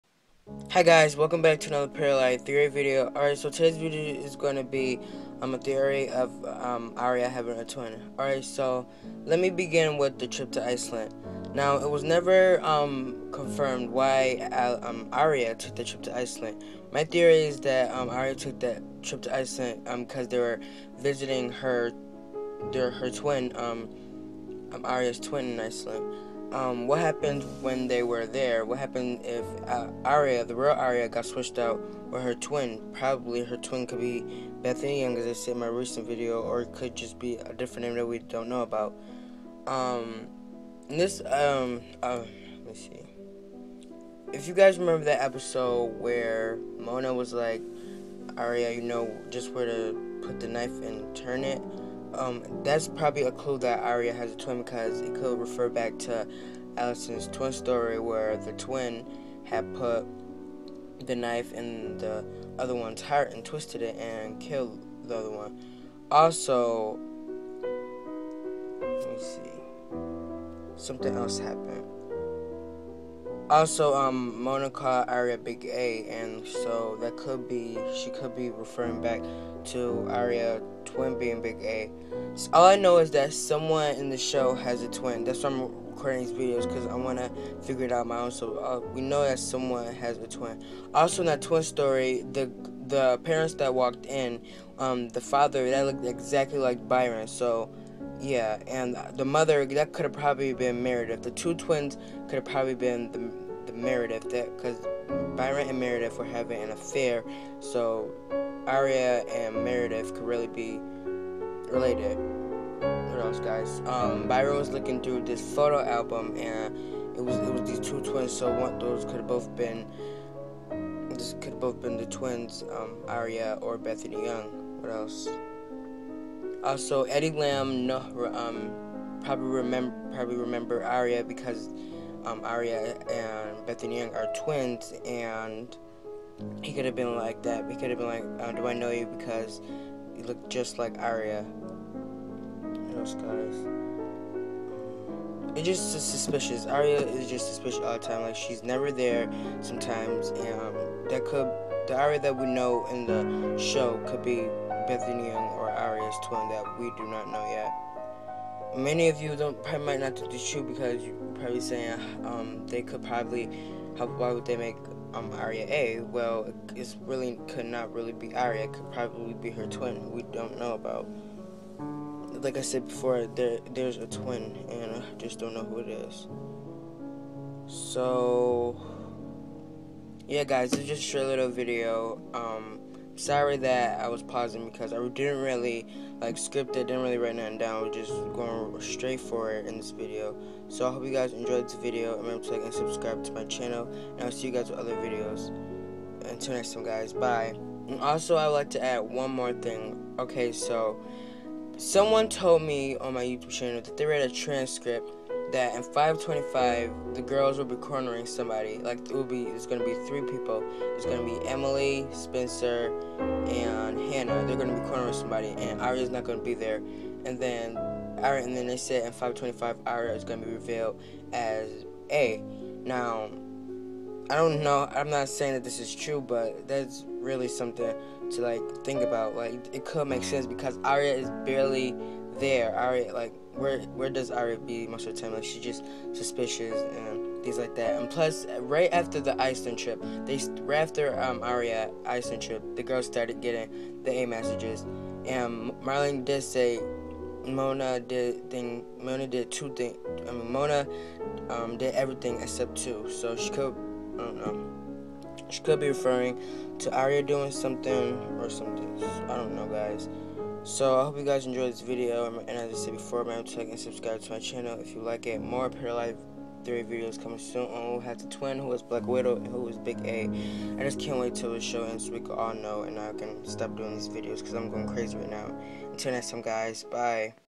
Pretty Little Liars Theory | Arias Twin + Mysterious Trip To Iceland ?!?!?!
More will be coming soon. Until me  time guys ... See you later. -A.  Tell me what u think in the comments.